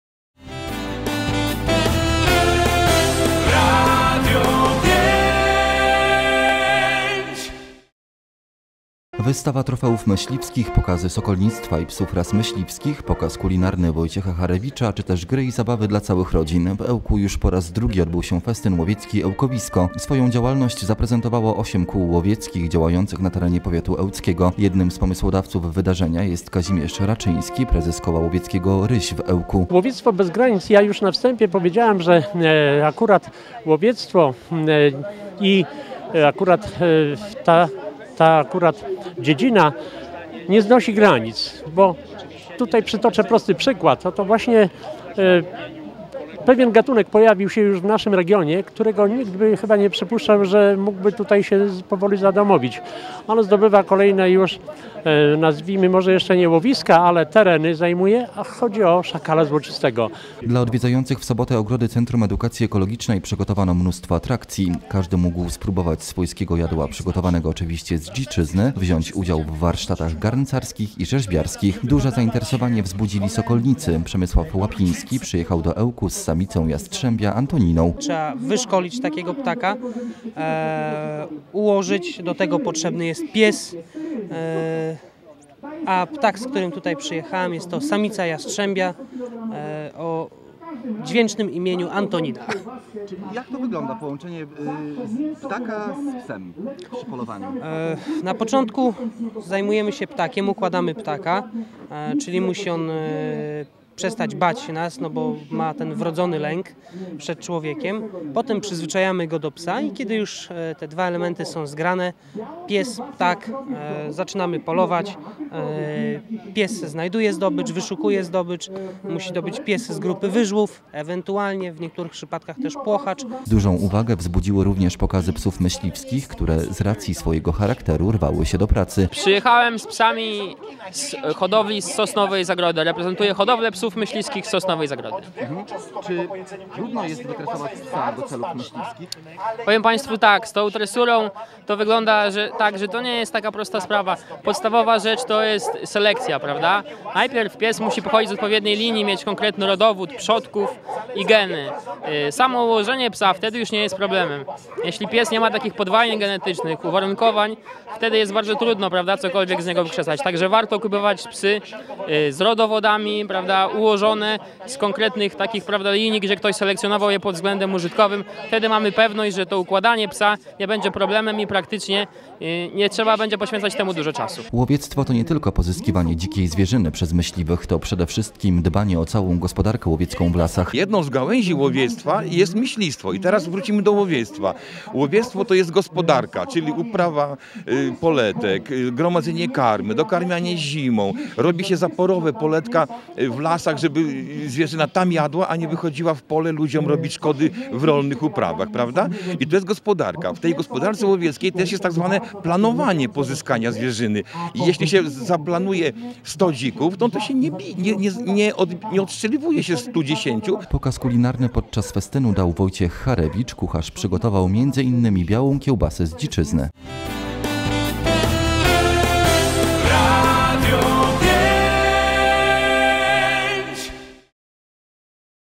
Festyn Łowiecki Ełkowisko
Wystawa trofeów myśliwskich, pokazy sokolnictwa i psów ras myśliwskich, pokaz kulinarny Wojciecha Charewicza czy też gry i zabawy dla całych rodzin. W Ełku już po raz drugi odbył się Festyn Łowiecki – Ełkowisko. Swoją działalność zaprezentowało 8 kół łowieckich działających na terenie powiatu ełckiego.

Jednym z pomysłodawców wydarzenia jest Kazimierz Raczyński, prezes Koła Łowieckiego „Ryś” z Ełku. Jak mówi Radiu 5 organizator, celem festynu jest edukacja proekologiczna łowiectwa i rozpowszechnianie wśród mieszkańców mało znanej kultury i tradycji myśliwskiej.